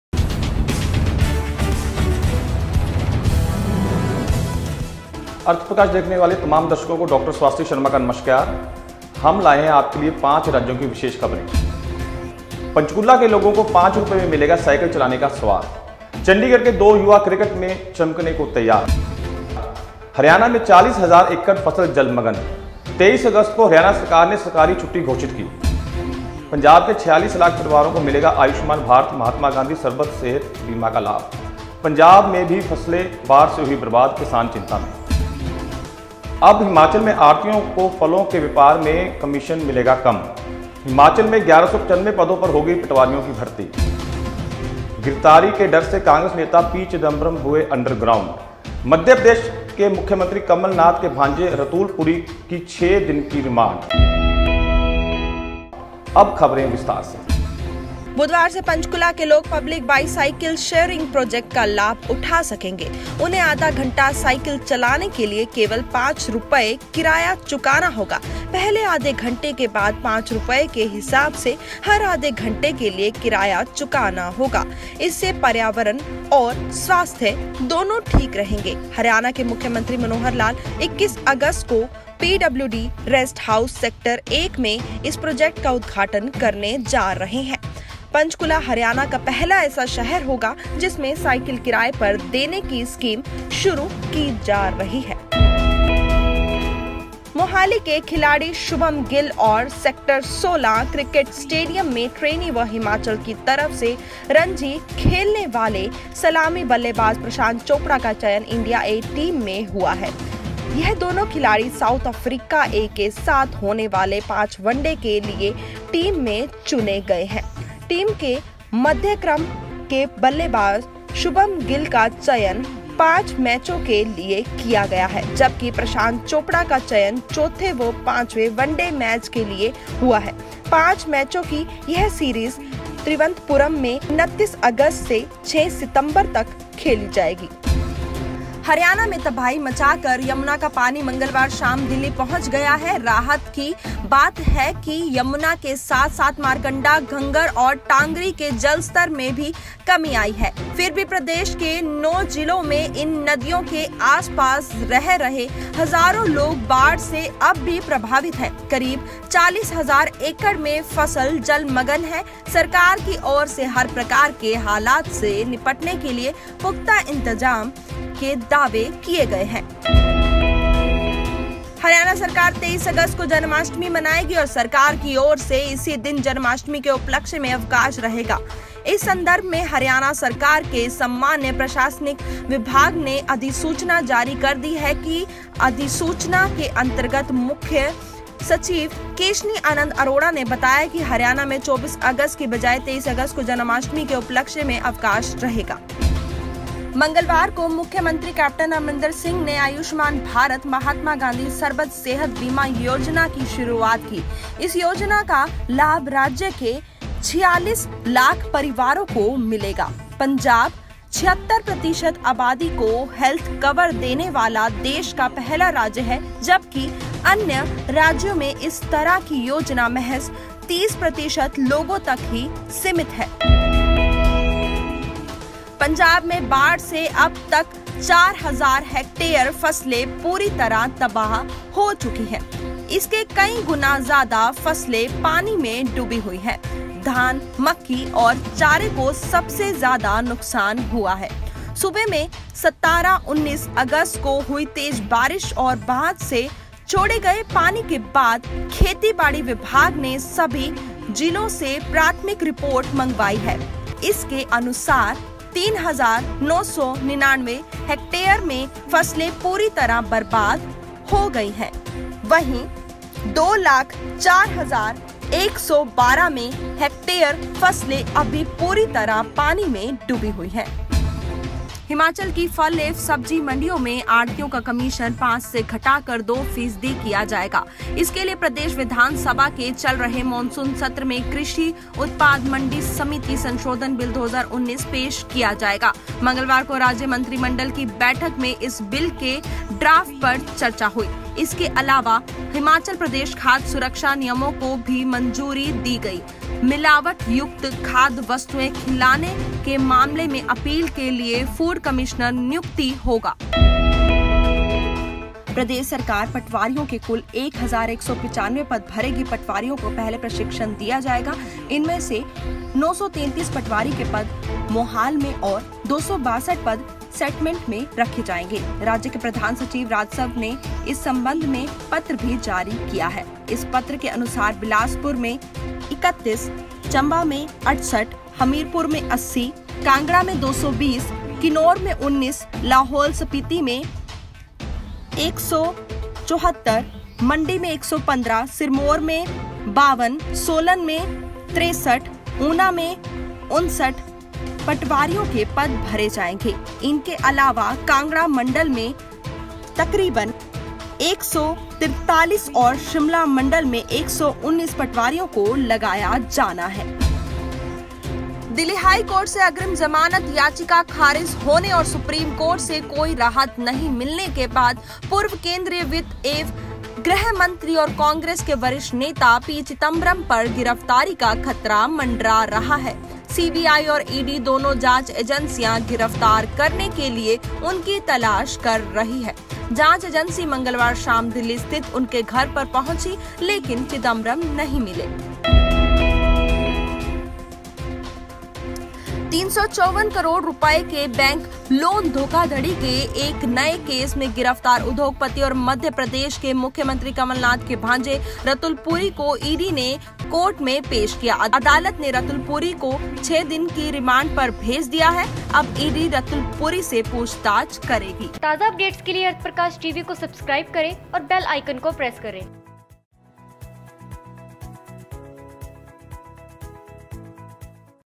News Bulletin : पांच राज्यों की बड़ी खबरें देखते रहिये अर्थ प्रकाश टीवी
पंचकूला के लोगो को 5 रुपये में मिलेगा साइकिल चलाने का स्वाद
चंडीगढ़ के दो युवा क्रिकेट में चमकने को तैयार
हरियाणा में  40 हजार एकड़ फसल जलमग्न
23 अगस्त को हरियाणा सरकार ने सरकारी छुट्टी घोषित की
पंजाब के 46 लाख परिवारों को मिलेगा आयुष्मान भारत-महात्मा गांधी सरबत सेहत बीमा का लाभ
पंजाब में भी फसलें बाढ़ से हुई बर्बाद, किसान चिंता में
अब हिमाचल में आढ़तियों को फलों के व्यापार में कमिशन मिलेगा कम
हिमाचल में 1195  पदों पर होगी पटवारियों की भर्ती
गिरफ्तारी के दर से कांग्रेस नेता  पी. चिदंबरम हुए अंडरग्राउंड
मध्यप्रदेश के मुख्यमंत्री कमलनाथ के भांजे  रतुल पुरी की छह दिन की रिमांड